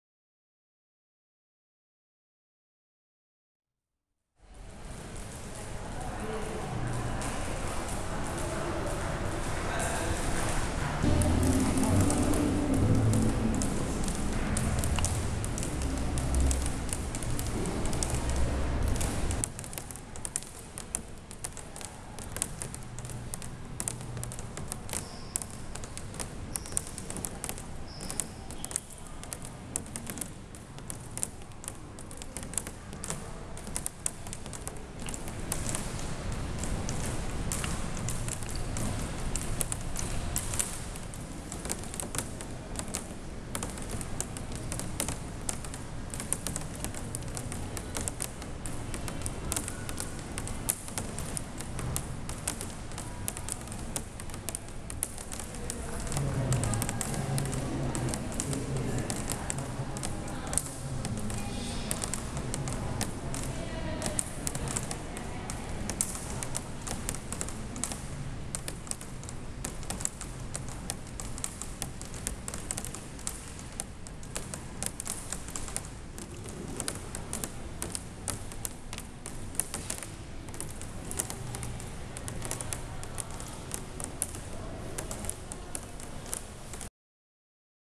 Exform, at DOJIMA River Biennale 2011, Osaka Japan
An installation work 'Exform' produced by artist IKEDA Kosuke and architect unite HARAGUCHI Kei+MIKI Keigo, symbolizing living organisms by droplets and hydrological circulation.
Dojima River Biennale 2011 held this summer at the Dojima River Forum, Osaka city. It was the second event in the series.